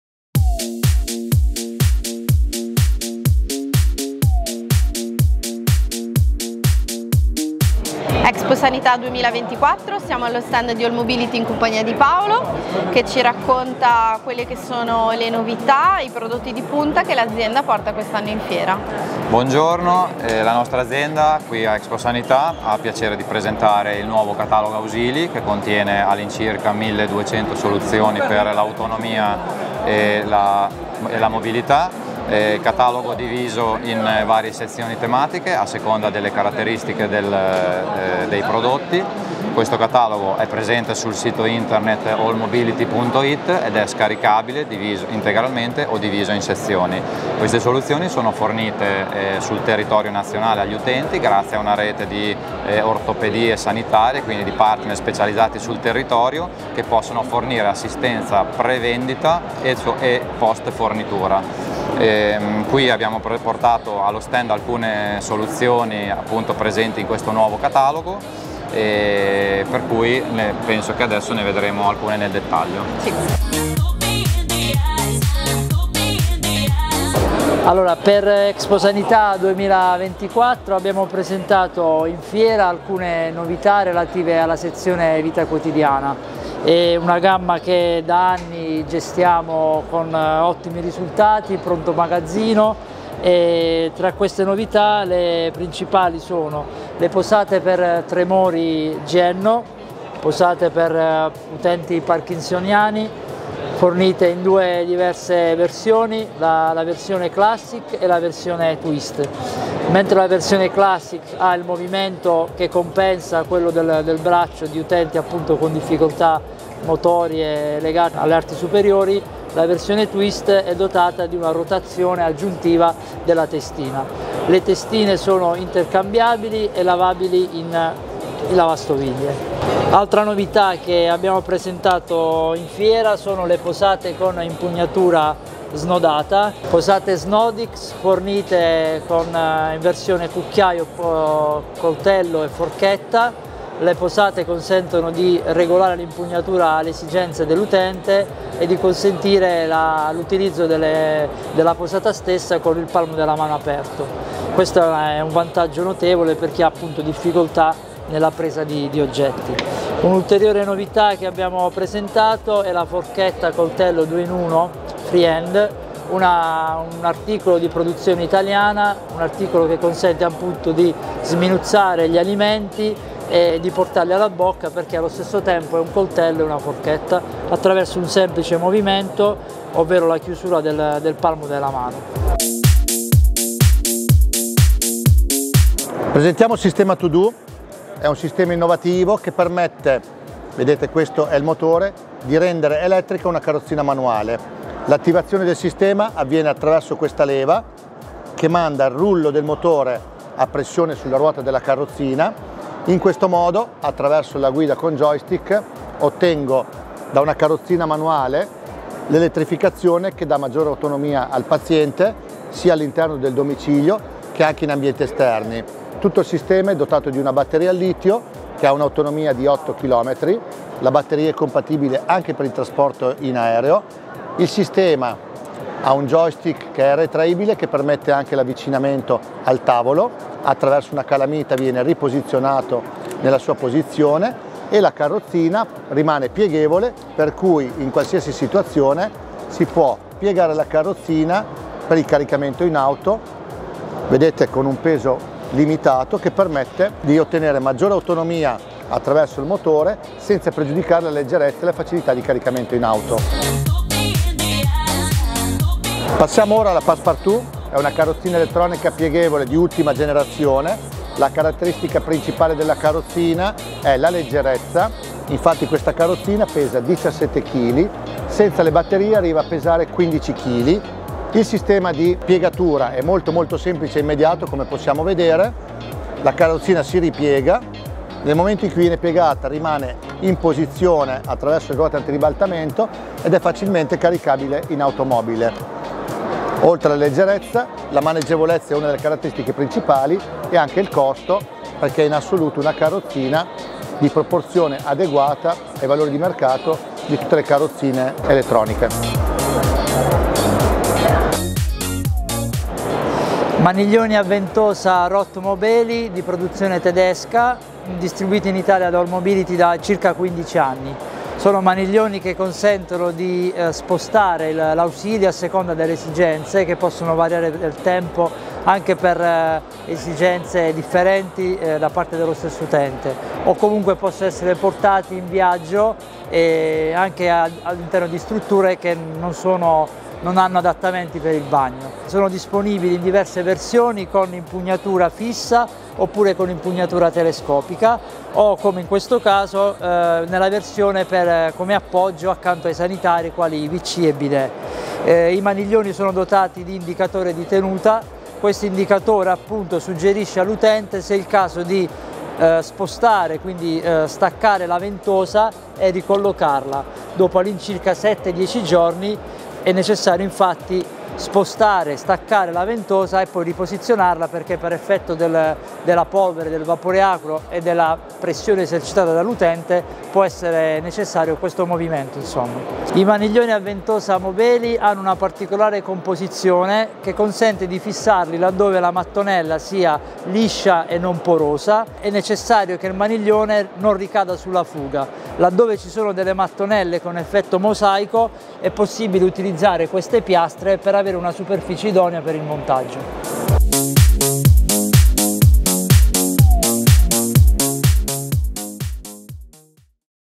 Alcuni degli ausili presentati ad Exposanità 2024
Un video della nostra presenza ad Exposanità 2024.
Posate e ausili per la terapia Occupazione, carrozzina elettronica leggera Passpartu, sistema per trasformare una carrozzina manuale in elettronica SIMPLY TO DO, maniglioni a ventosa senza foratura MOBELI